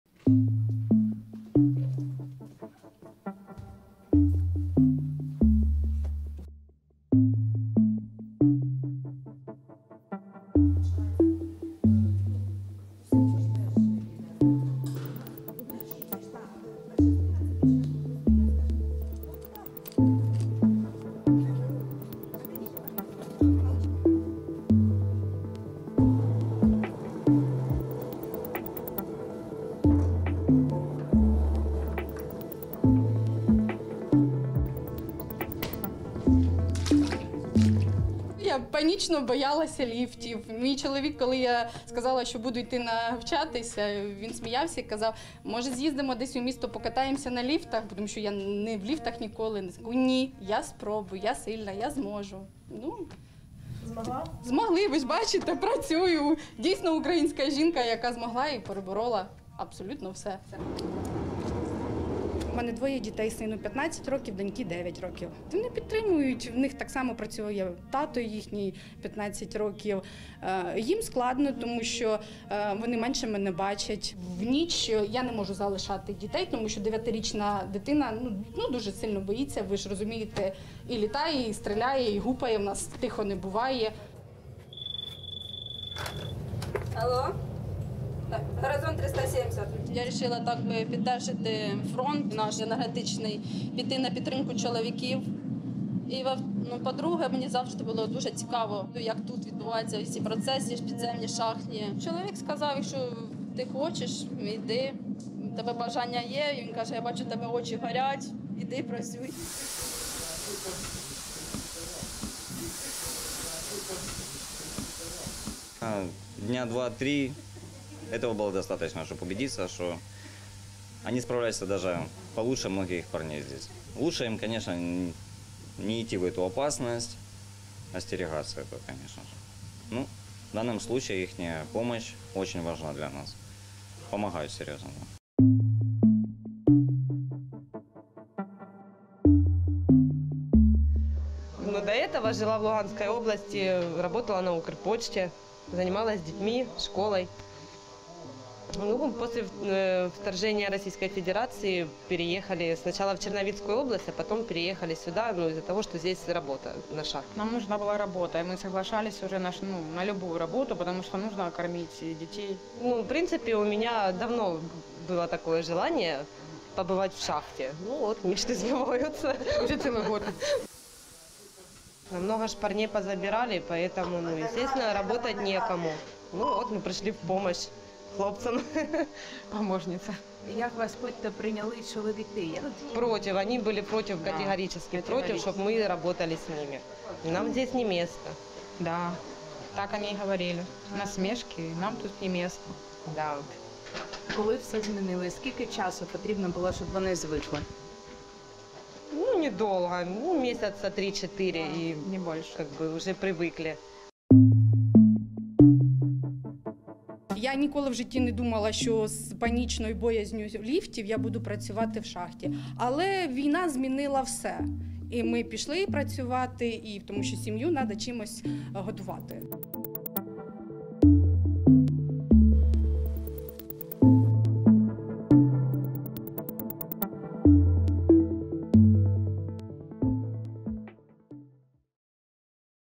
Украинки ги надминуваат стравот и предрасудите за рудниците да останат отворени
Шестотини рудари беа регрутирани во украинската армија од рудник кој е 370 метри под земјата, во близина на Павлоград, во областа Днепропетровск. Жените пополнија многу од празните места откако владата го смени законот за да им дозволи да работат под земја, во обид рудниците да продолжат да работат.